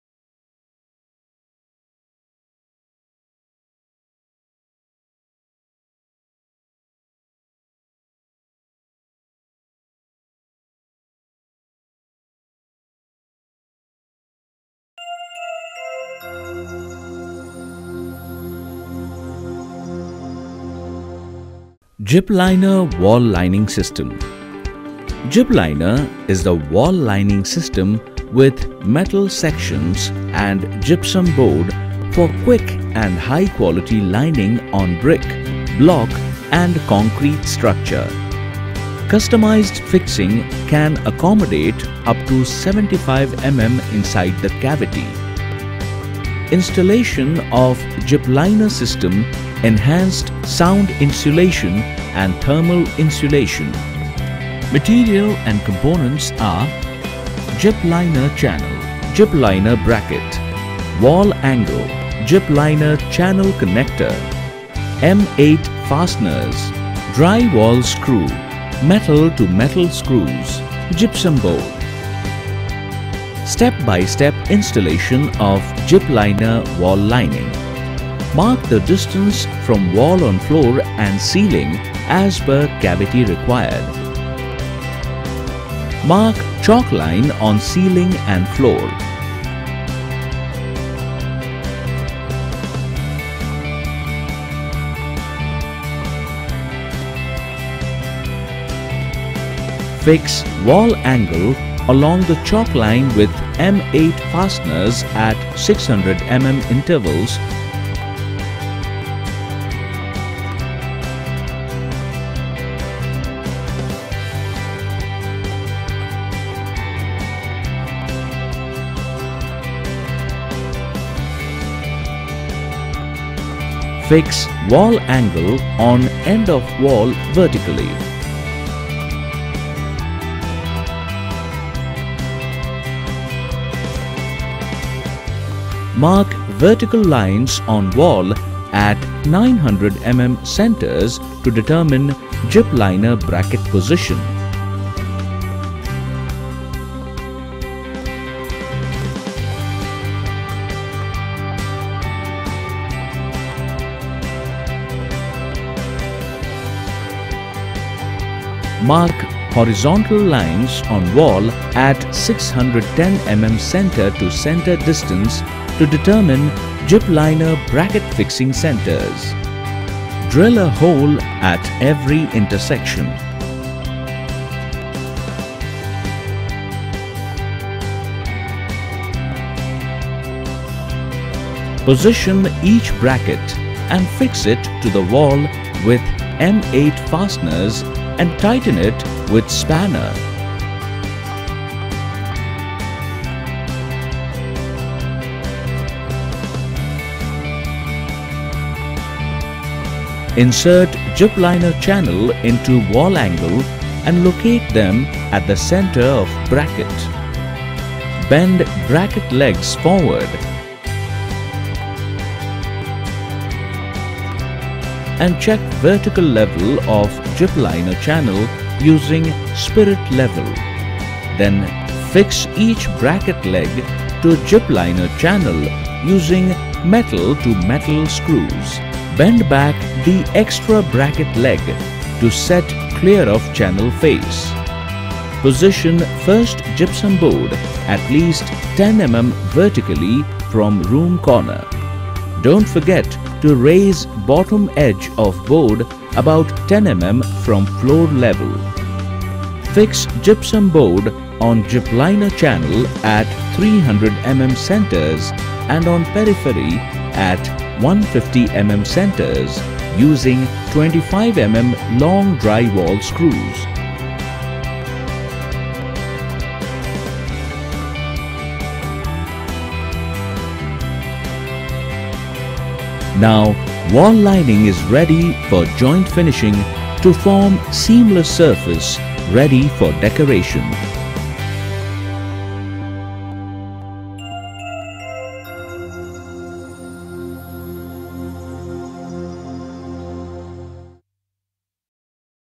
Gypliner Wall Lining System by Gyproc | High Quality Wall Lining System
Gypliner is the wall lining system with metal sections and Gypsum board for quick and high-quality lining on brick block and concrete structure. Customized fixing can accommodate up to 75 mm inside the cavity. Installation of deployment system enhanced sound insulation and thermal insulation.

00:00 - Introduction
00:22 - Gypiner wall lining system
00:56 - Materials and Components
01:14 - Step by Step Installation of Gypliner wall lining
02:04 - Fixing of Wall Angle Vertically
02:40 - Fixing of Gypliner Bracket
03:20 – Placing of Gypliner Channel
03:35 – Levelling of Channel
03:55 – Board Fixing
04:33 – Wall Lining is ready for Joint Finishing to form seamless surface ready for decoration